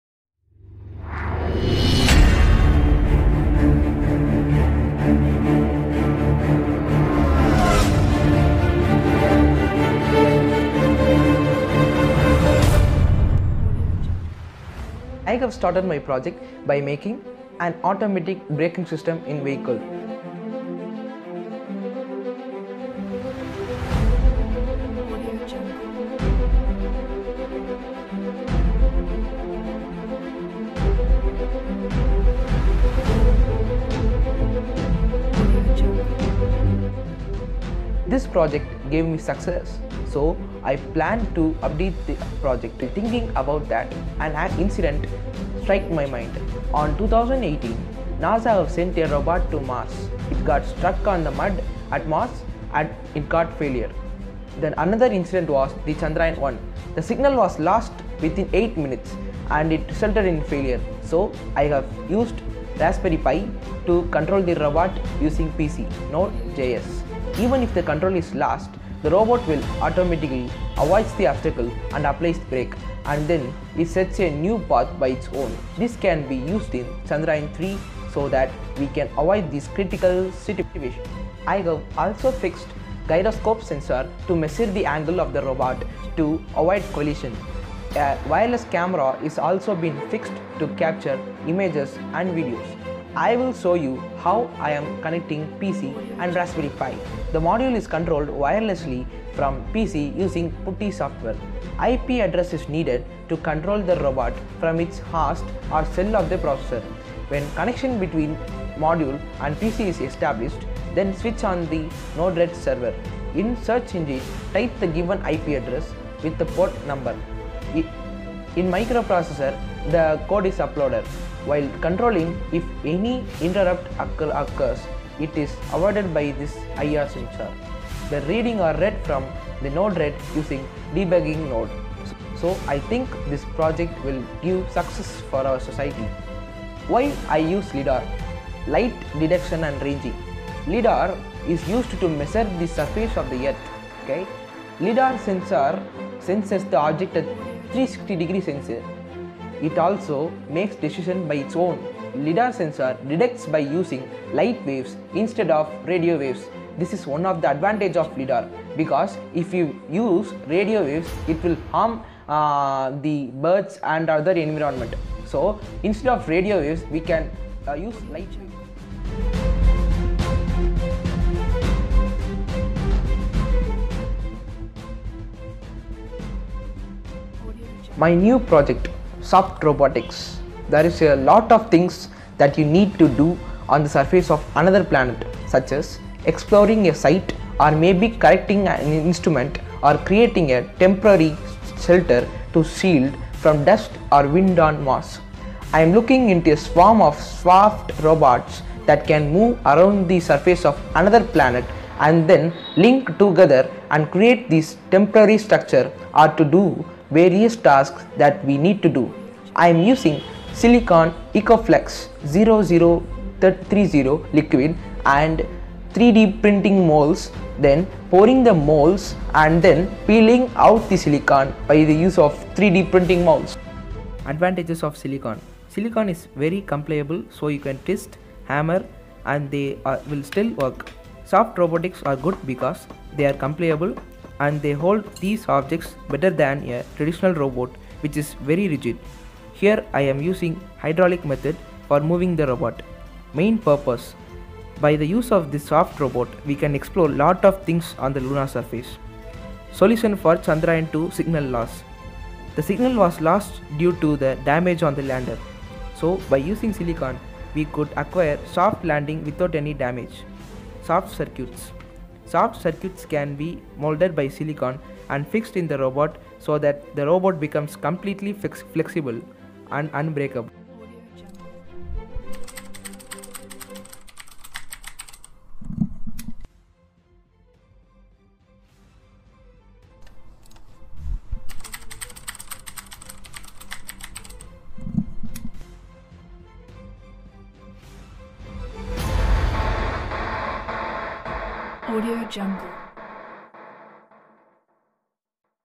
High Tech Space Robot
The project was started by making an automatic breaking system in vehicles it avoids the obstacle and vehicle which comes to hit. It is updated to a robot which avoids the obstacles and apply the break using gyroscope sensor, IR sensor and raspberry Pi. Here silicon is used as it is very compliable. We can make soft landing of rover on the lunar surface without any damage. It can explore lot of things in space which cannot be done by traditional robots.